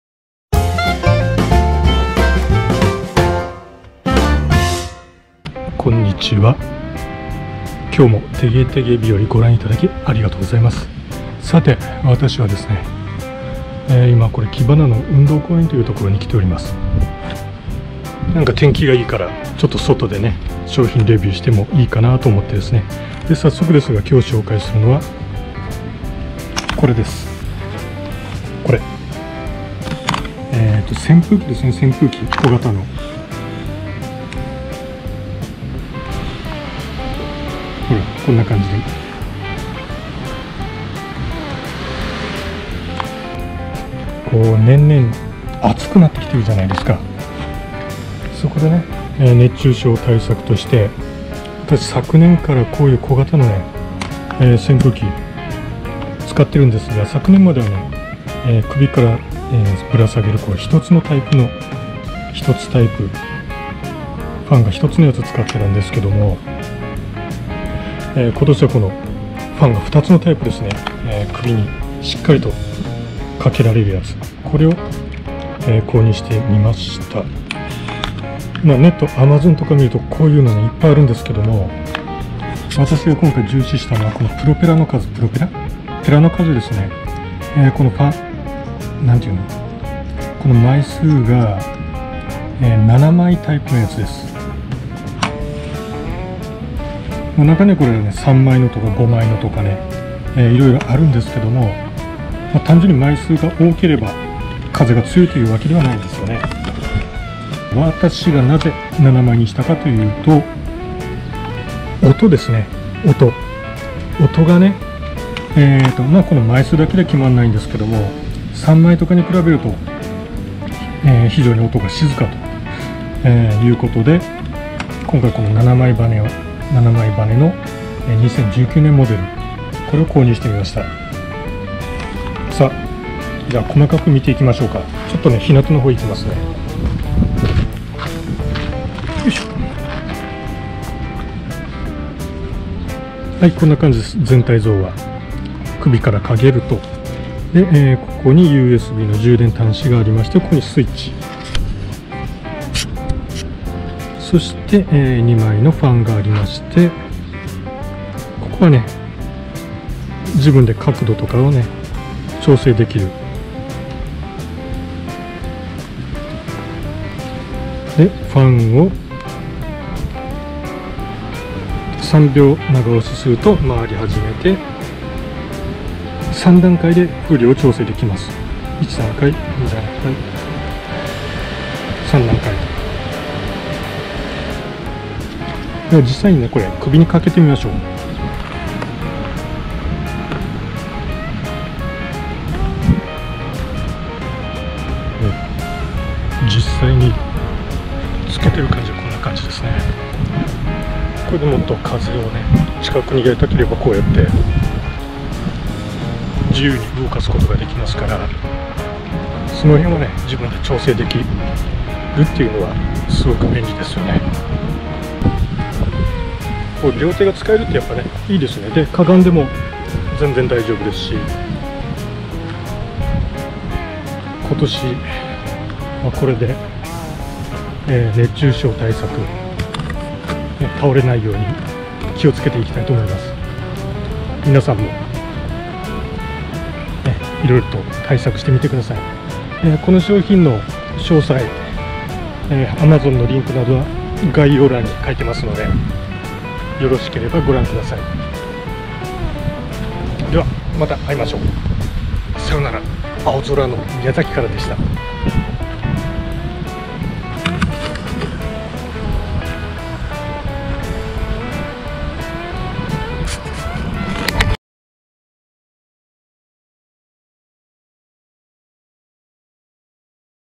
【レビュー】首かけモバイル扇風機2019バージョン：熱中症対策に最適！
今年大人気の「首かけモバイル扇風機」２０１９年バージョンを買ってみた。大きな違いは羽が７枚になったこと。これで作動音が小さくなっているらしい。
熱中症対策のどうですか？

紹介した商品はこちら
■Rakoko 扇風機首掛け ハンズフリー扇風機  7枚羽根

てげてげ日和